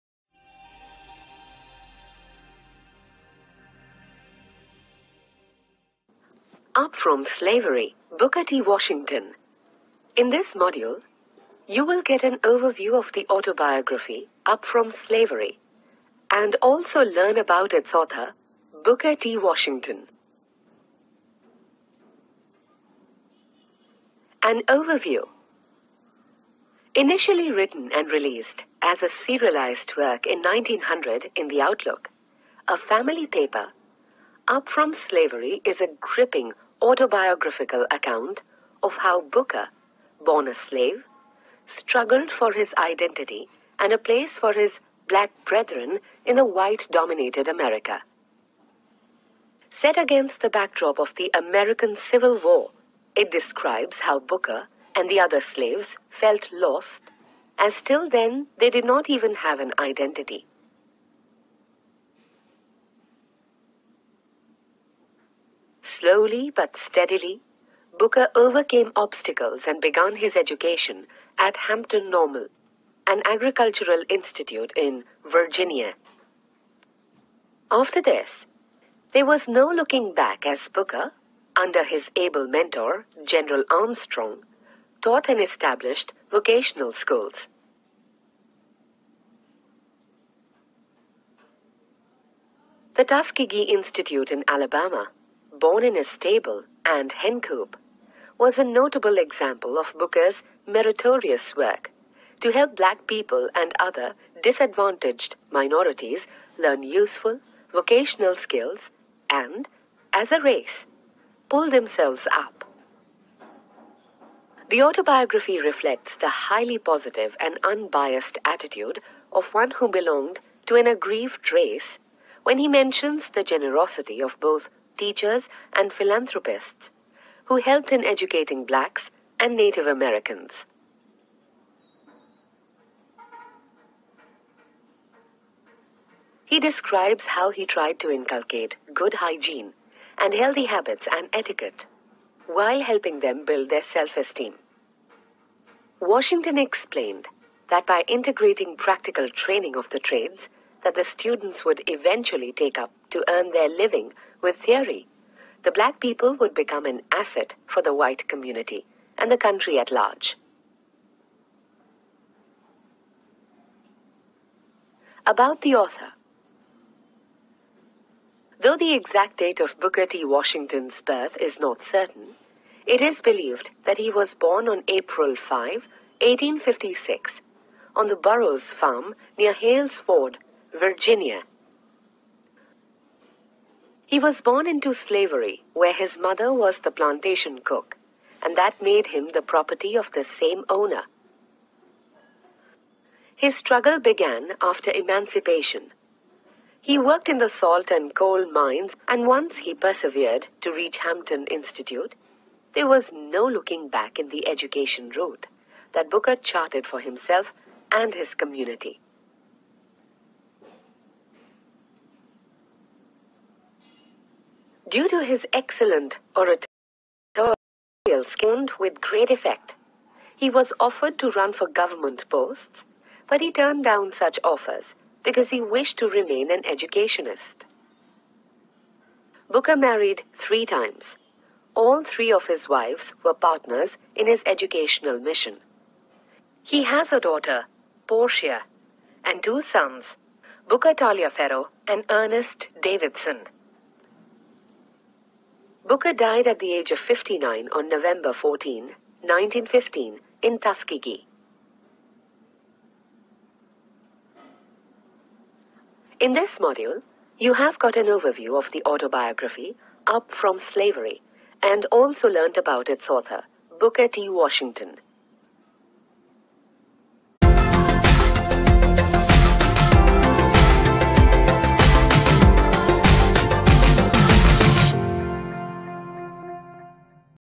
Up From Slavery - An Overview, Summary And About The Author - CBSE CLASS 11
Up From Slavery - Booker T Washington - An Overview, Summary And About The Writer!
CBSE CLASS 11 LONG READING TEXTBOOK

From Team
QualityEdu